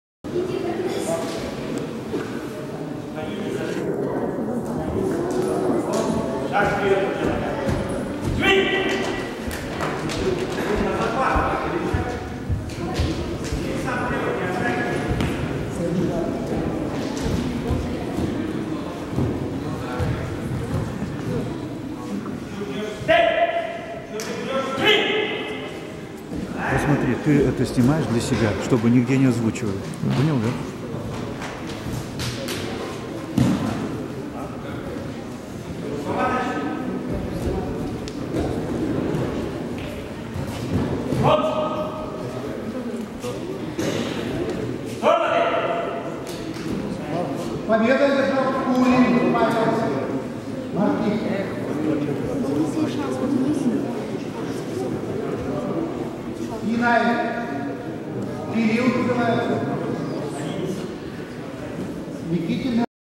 денис рожков павел кулин
Турнир по дзюдо посвящённый памяти Мастера спорта СССР , призёра первенства СССР Тахира Гайсиевича Мифтахова.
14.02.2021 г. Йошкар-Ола